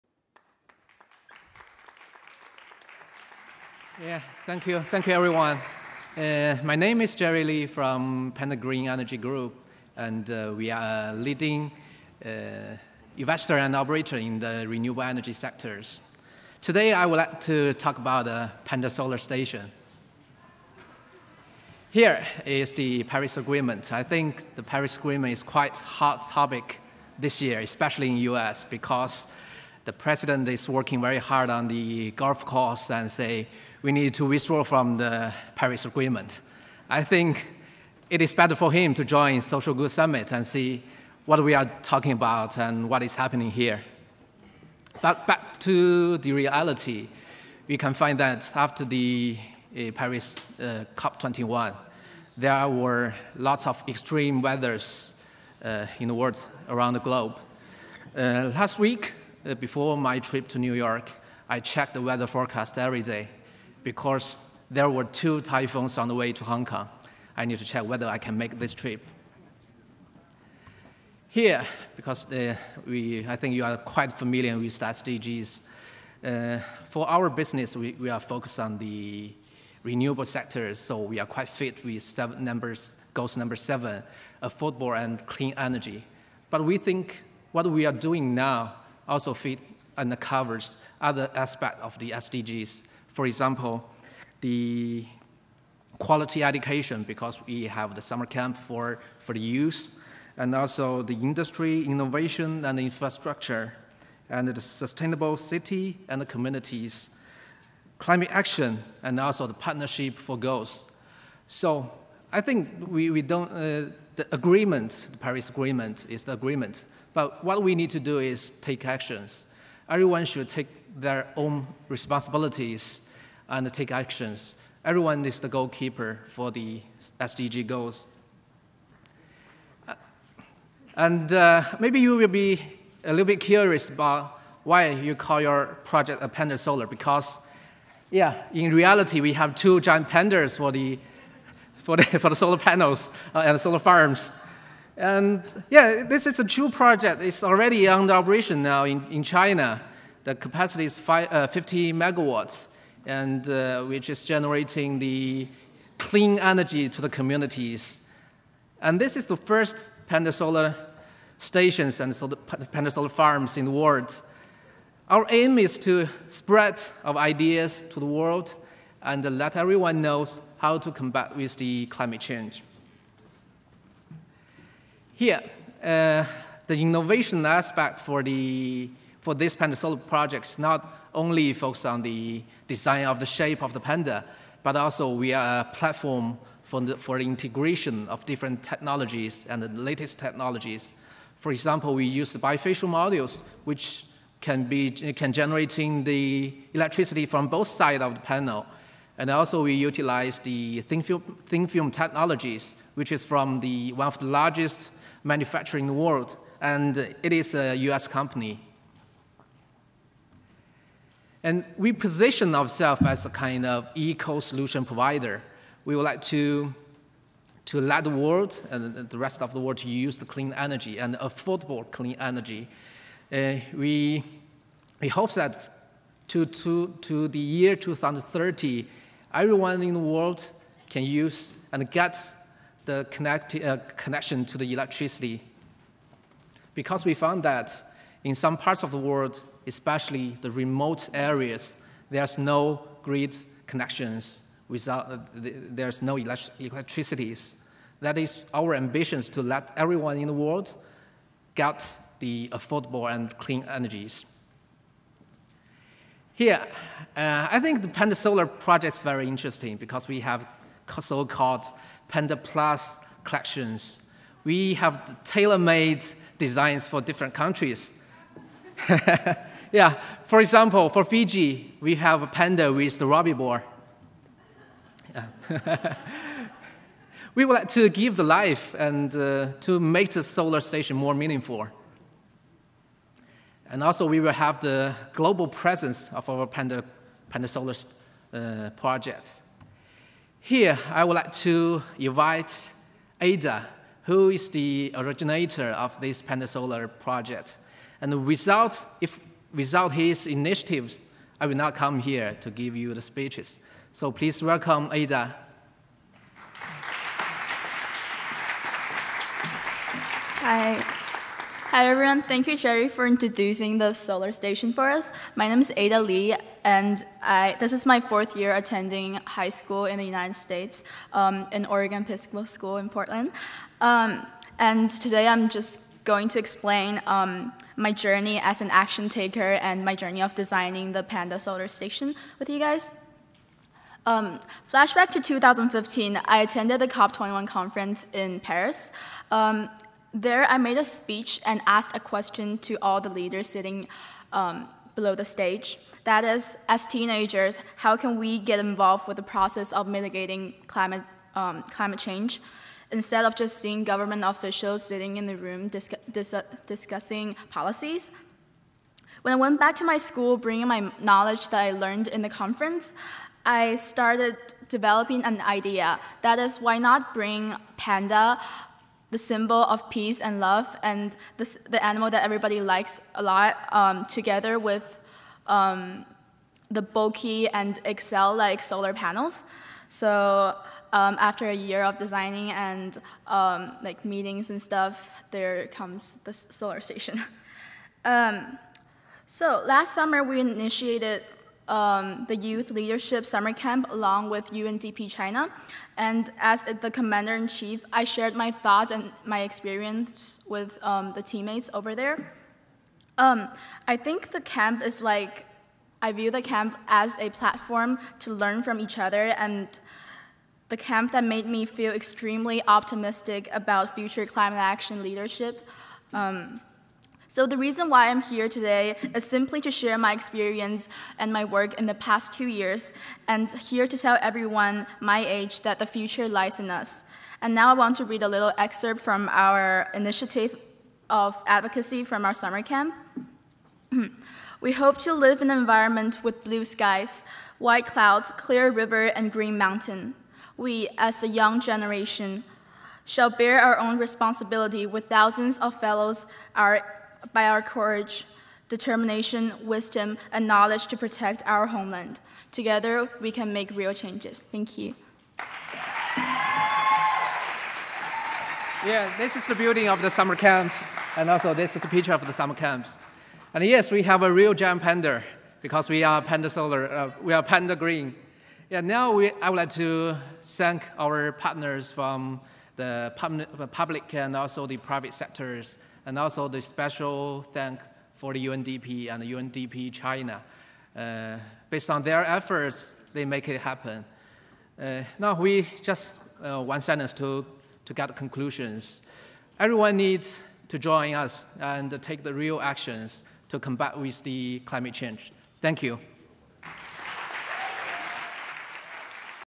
Panda Green Energy Group and the Future of Renewable Energy
Panda Green Energy Group's Jerry Li and Originator, Ada Li,  discuss the initiatives they are taking to make green energy affordable and available worldwide.